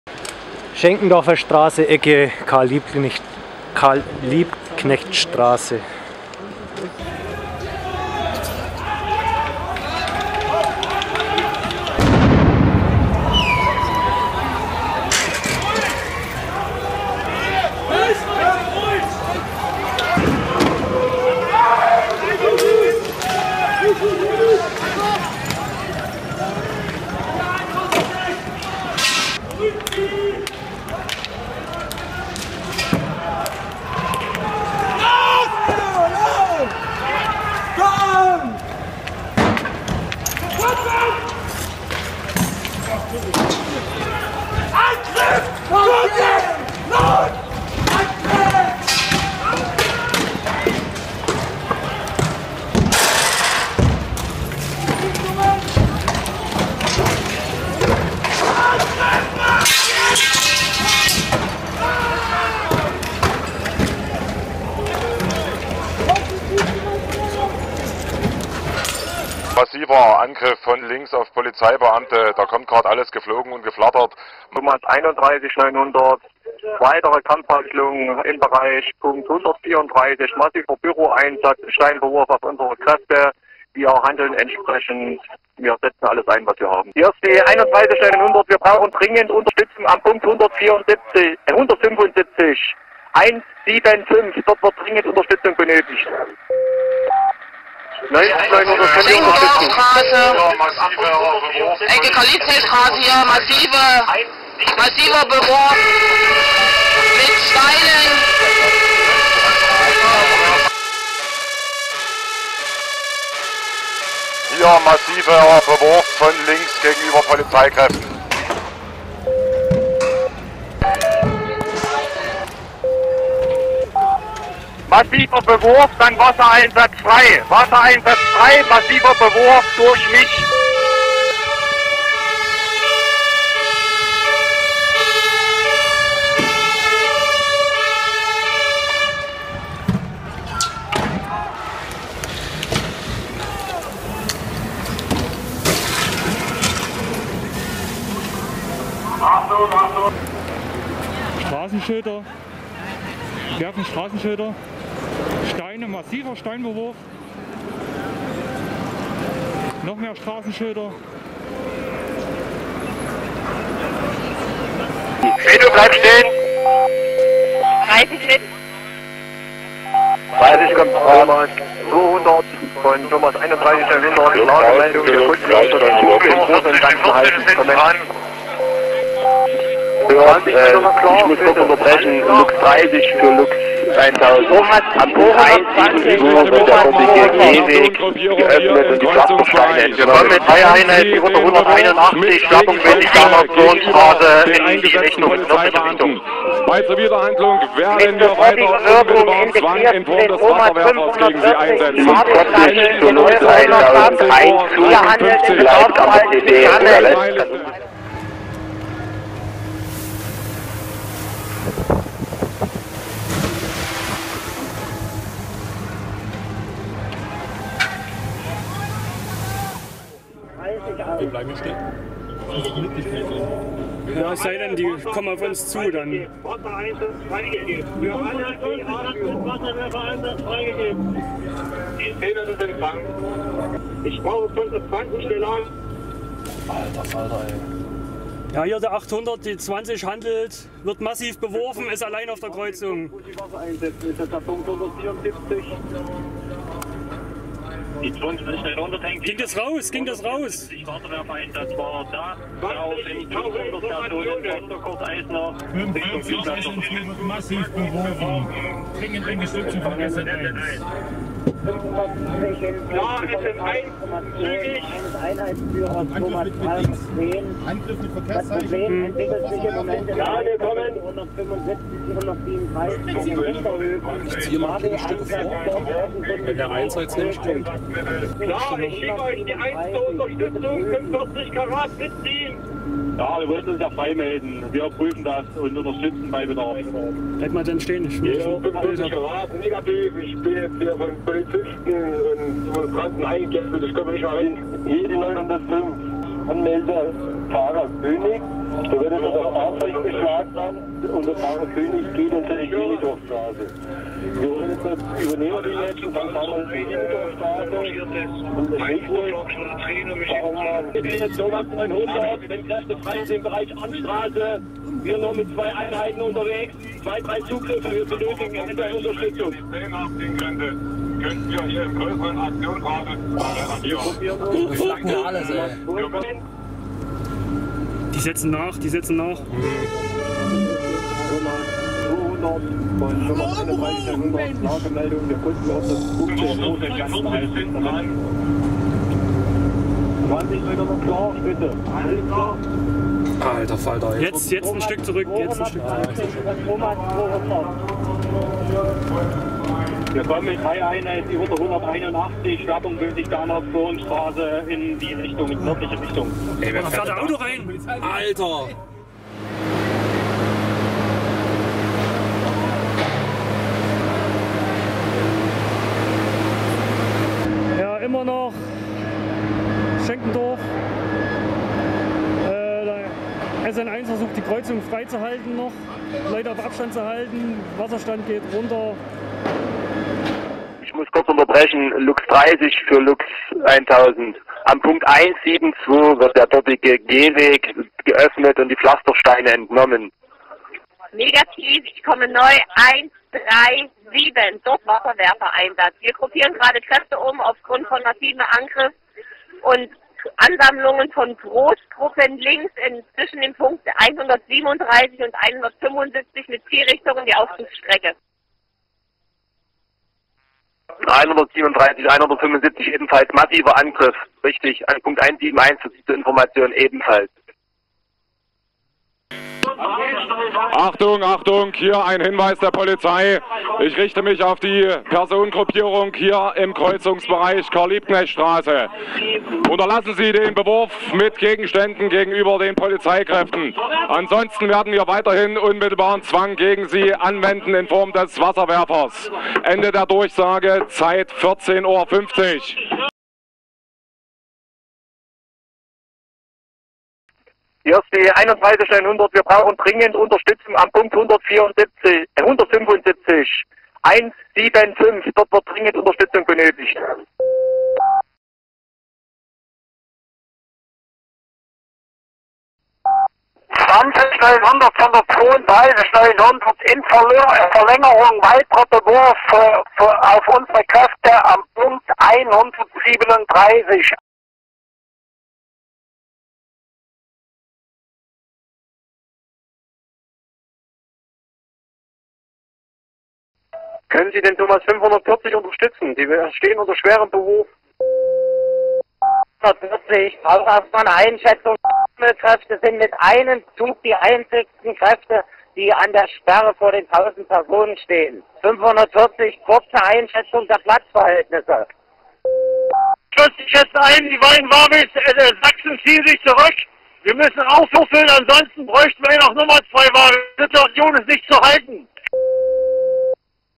LE1212 Schenkendorfstraße Angriff auf Polizei Leipzig 12.12.2015 14.50 Uhr
Die beiden Wawes Sachsen ziehen sich zurück. Wir müssen auffüllen, ansonsten, bräuchten wir hier nochmal 2 Wawes. Die Situation ist nicht zu halten.

Aus "Rückblick: Die Straßenschlacht… 4. Jan, 2017"
Den zweiten größeren Versuch gab es am Südplatz, hier entstanden die wohl eindrucksvollsten Bilder des Tages von einer sehr langen und heftigen Auseinandersetzung zwischen AntifaschistInnen und der Polizei. Ein erfolgreicher Durchbruch hätte direkt zum Endpunkt des Naziaufmarschs geführt, wobei die Polizei den Aufmarsch dort letztendlich nicht beendete, sondern die Neonazis lieber zum Ausgangspunkt zurück führte.

Für viele AntifaschistInnen war die Straßenschlacht am Südplatz die heftigste seit den neunziger Jahren in Leipzig.